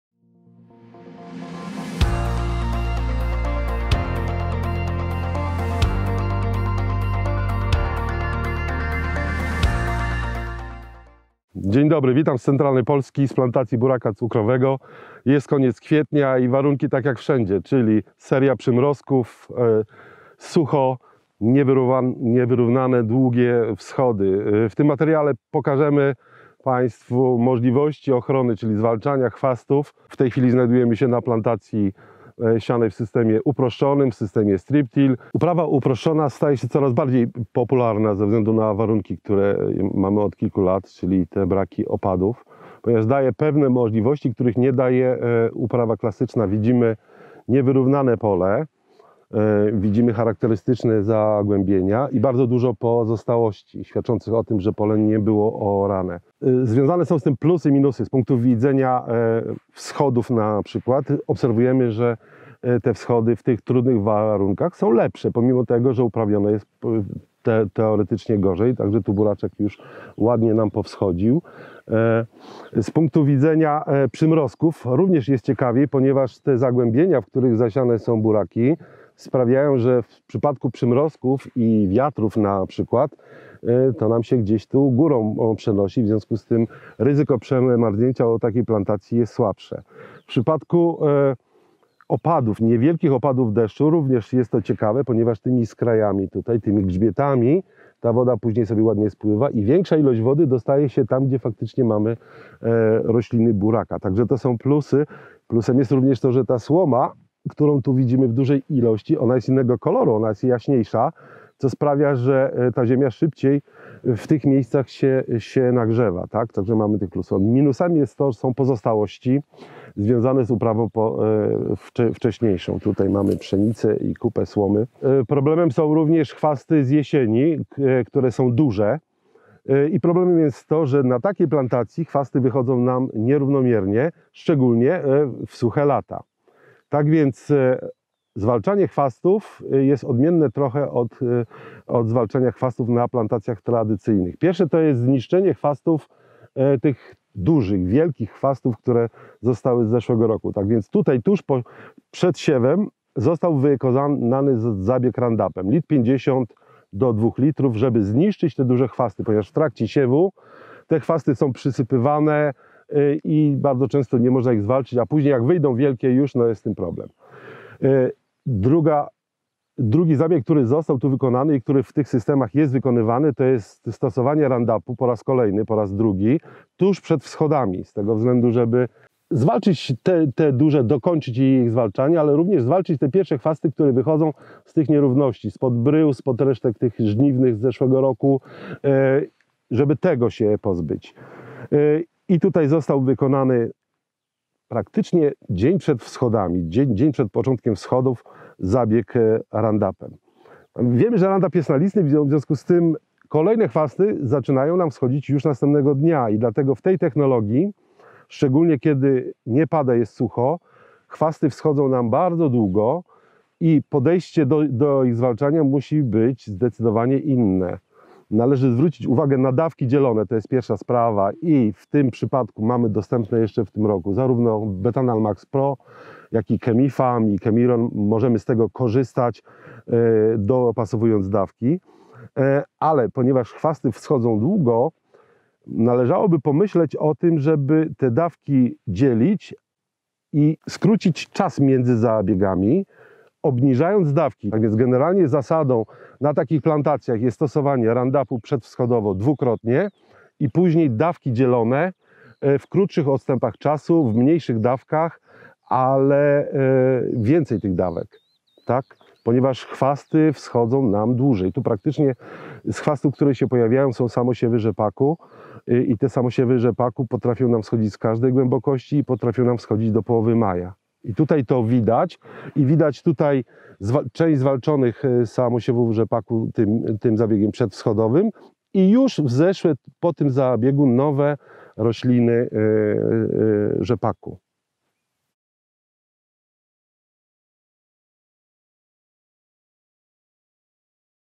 Zwalczanie chwastów w buraku cukrowym w technologii strip till
Uprawa pasowa jest w Polsce coraz bardziej popularna, ze względu m.in. na możliwość oszczędnego gospodarowania wodą glebową. Jednak system ten stawia przed rolnikami nowe wyzwania dotyczące m.in. skutecznej ochrony plantacji przed chwastami. W jaki sposób przeprowadzić oprysk herbicydowy, jakie preparaty dobrać i na co zwrócić uwagę podczas zabiegu ochrony, podpowiada Andrzej Żmijewski, doradca Bayer.